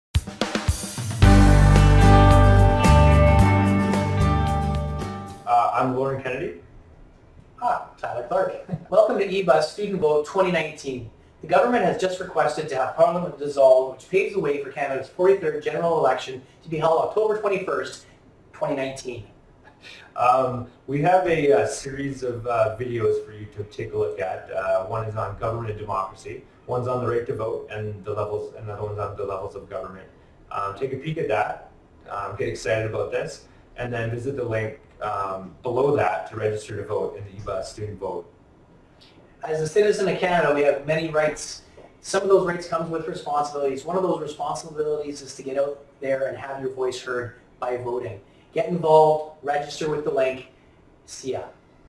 Ebus Academy Student Vote 2019
Two rad guys getting students jazzed about voting!